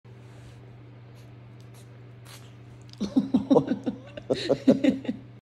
Skunk has a BIG SNEEZE!
Gizmo gives us big sneeze, blows himself back! haha!